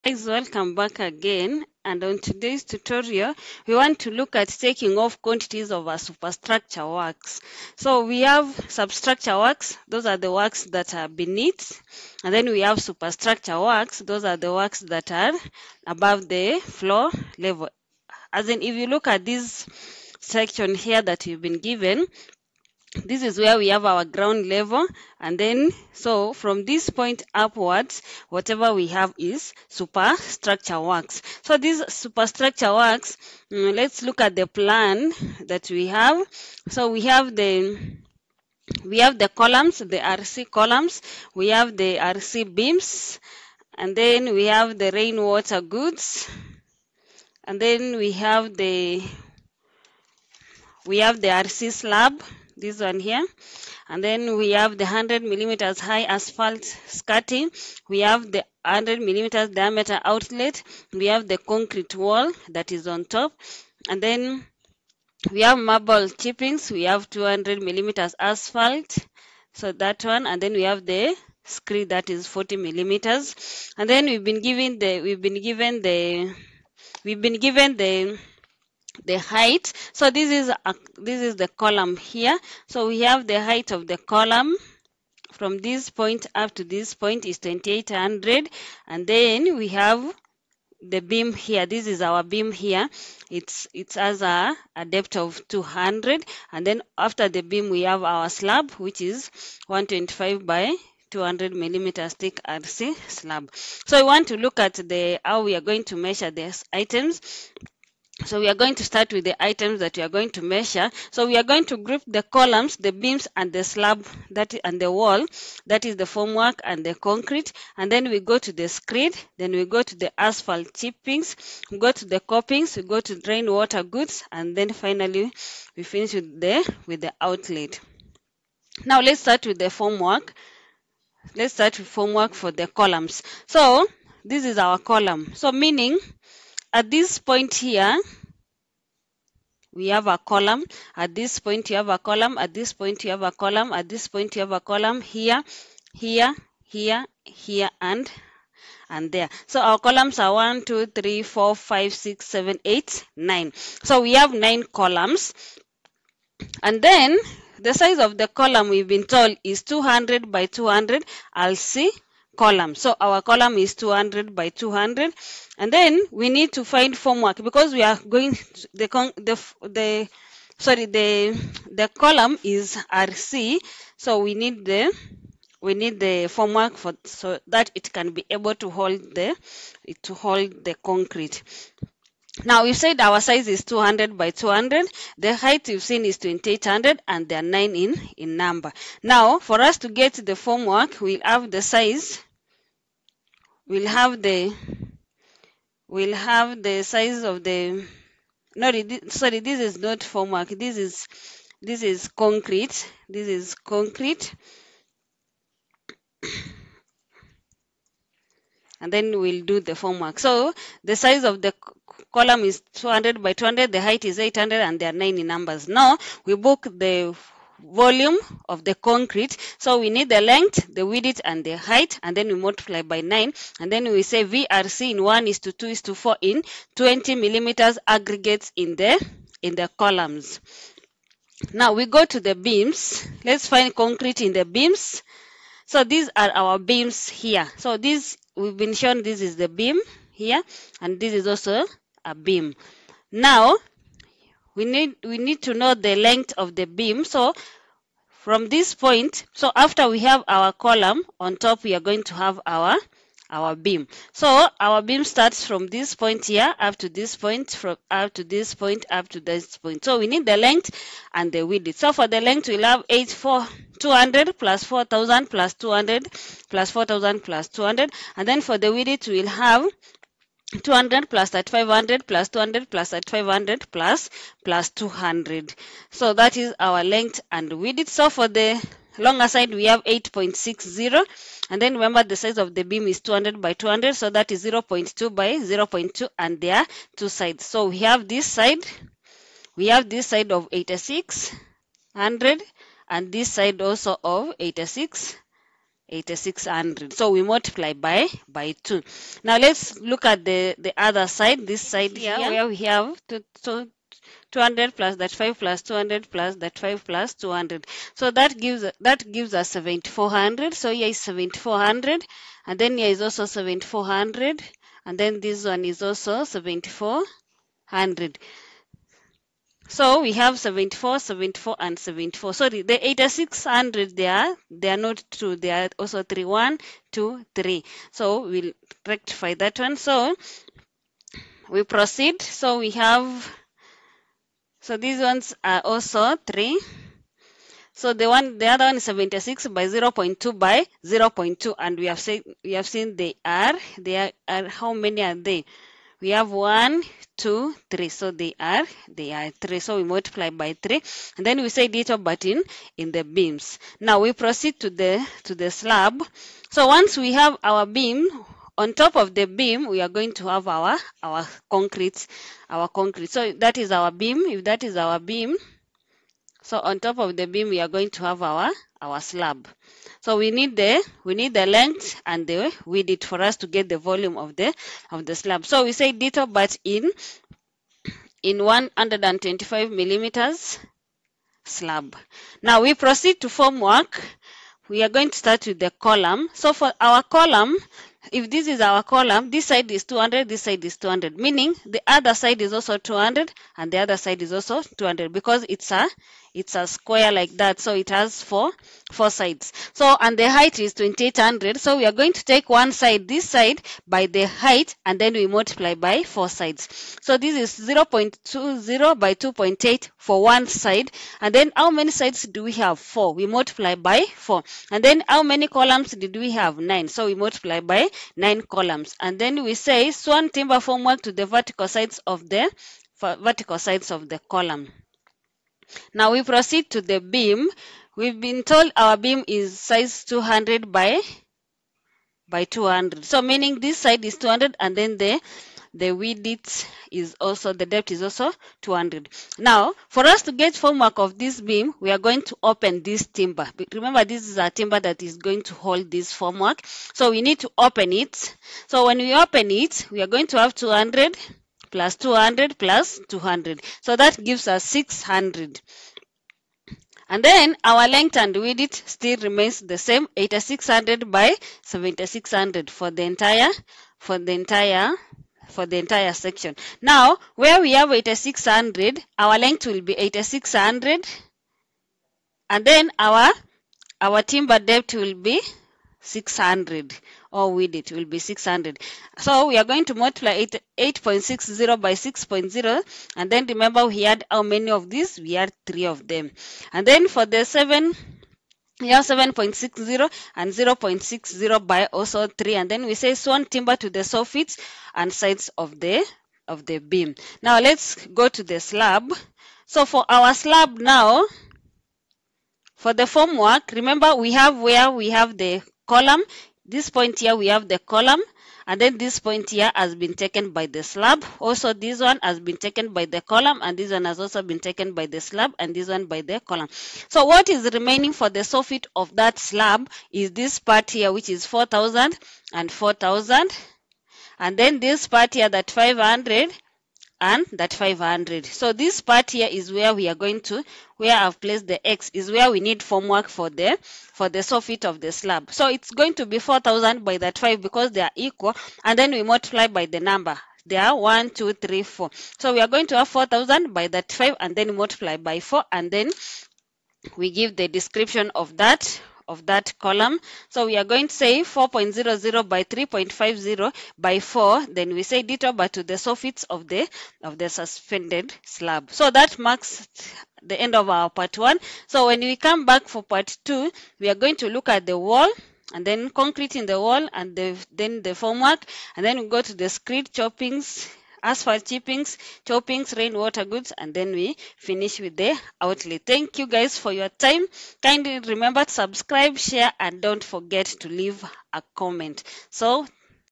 Taking off Quantities of Superstructure works| Measurements
Hello guys in this video we shall look at how to calculate quantities of superstructure works that is works which are above the ground level